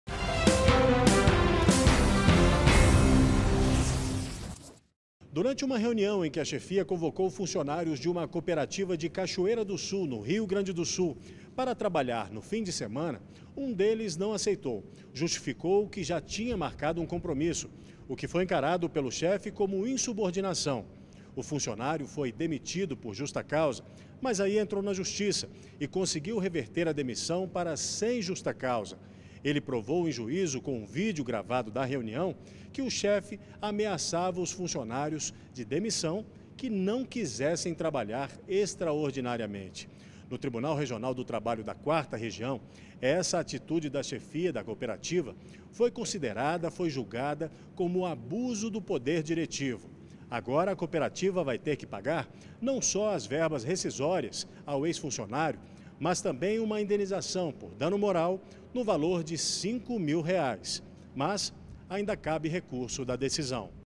Empregado obrigado a trabalhar no fim de semana vai receber indenização
EMPREGADO ENTRA NA JUSTIÇA CONTRA EMPRESA QUE O OBRIGOU A TRABALHAR NO FIM DE SEMANA. O FUNCIONÁRIO GANHOU A CAUSA. MAIS DETALHES COM O REPÓRTER ALEXANDRE MATULA.